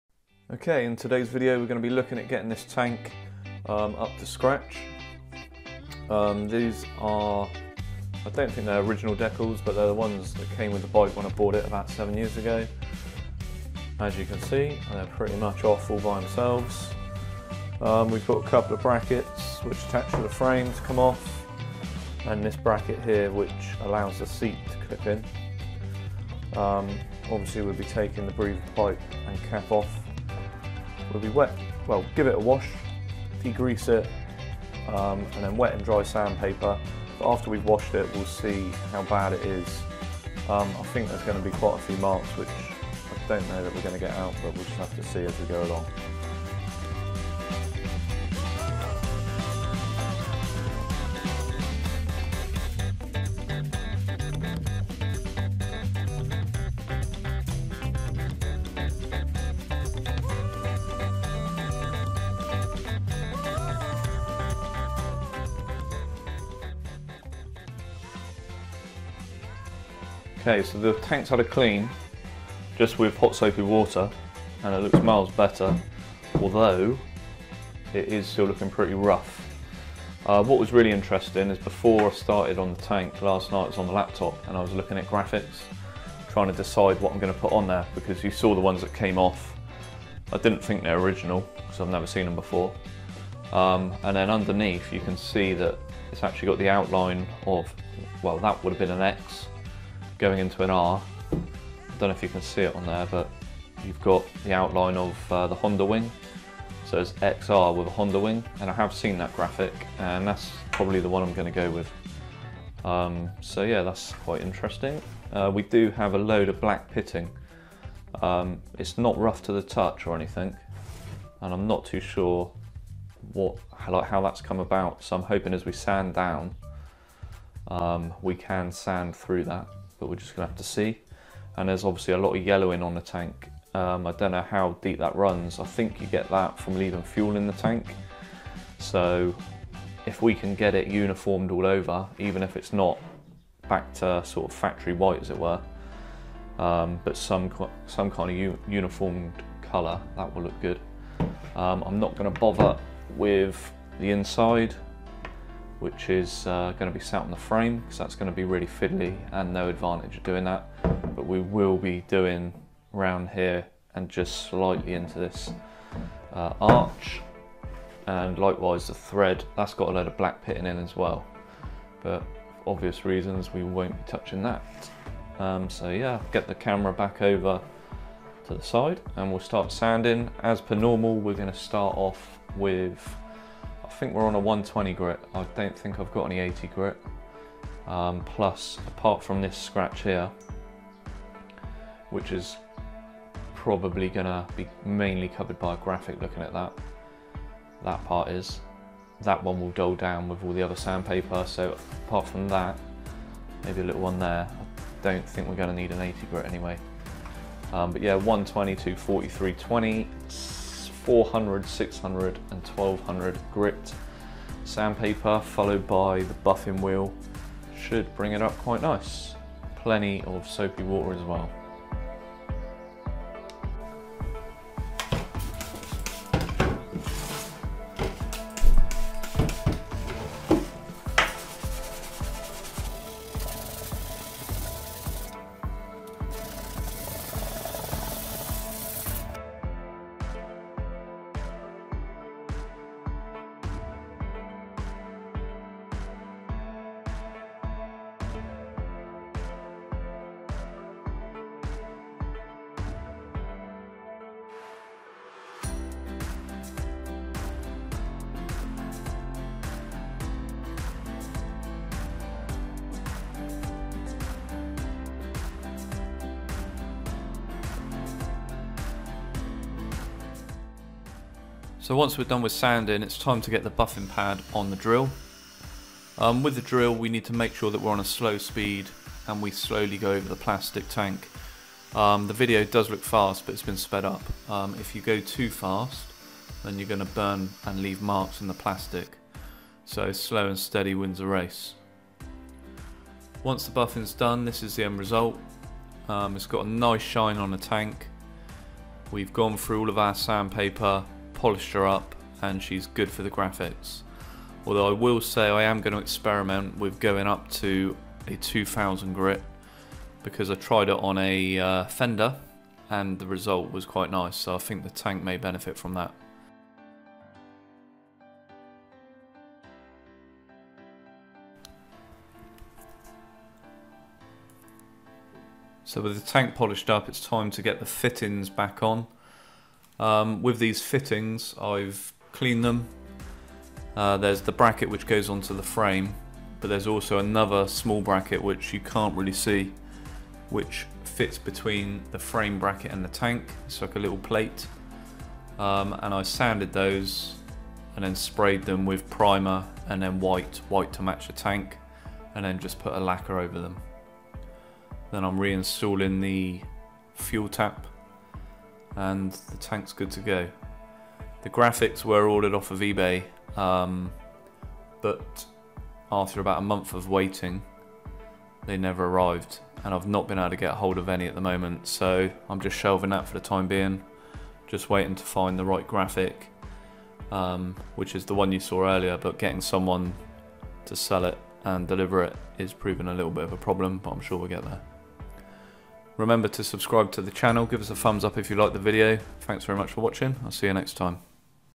XR Tank Refurb - Honda Dirt Bike Project
In this video you can see the process I went through to refurb the tank for the Honda XR250 project.